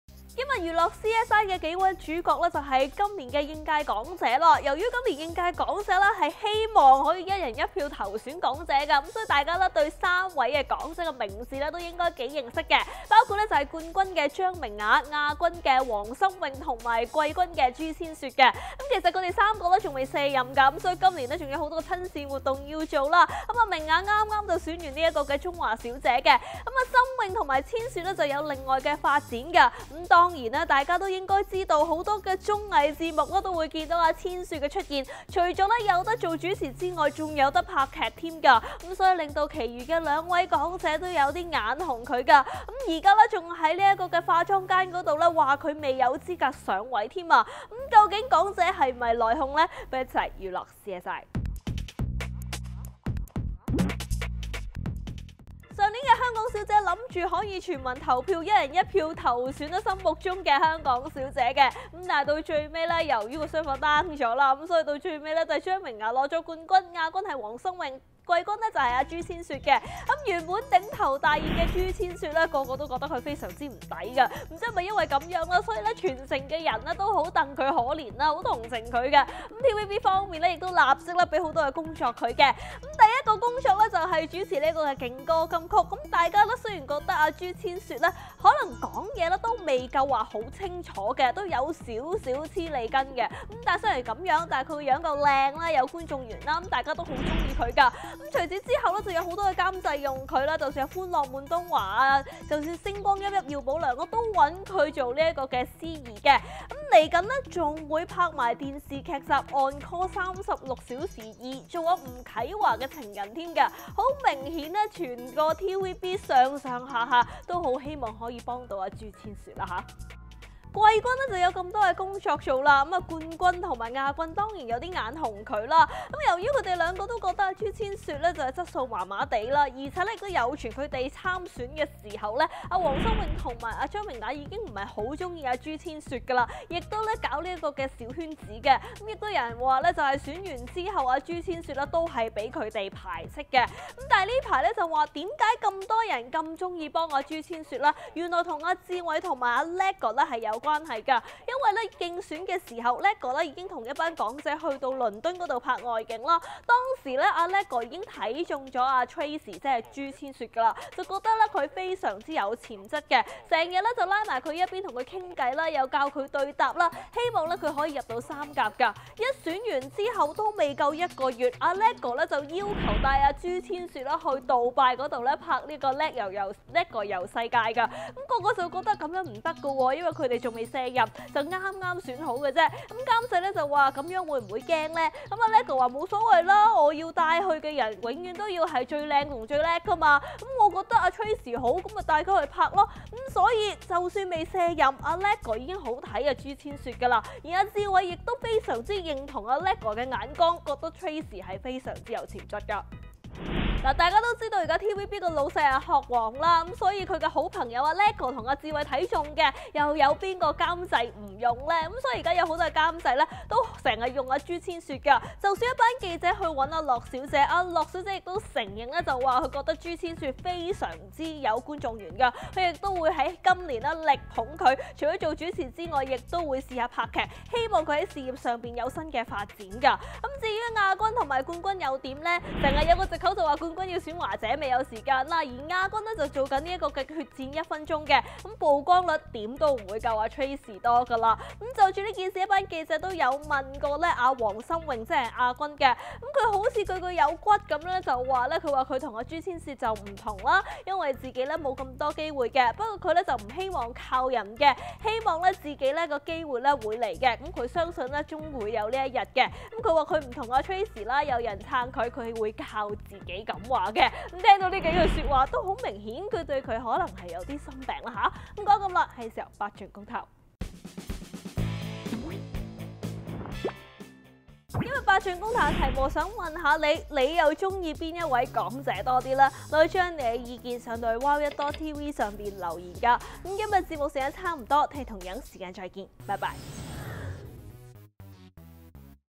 0226 八尽娱乐- 阿叻點名谷朱千雪 黃心穎話「冇人撐」有骨
你又鍾意邊位港姐多d？